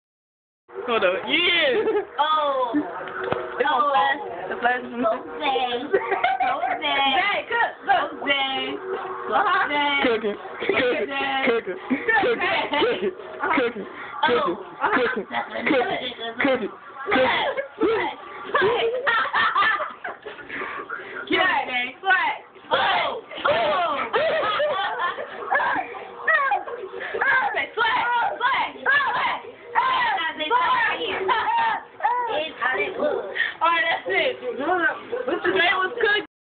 My little cousin cooking .. swagg!
1 year old cookin , lil b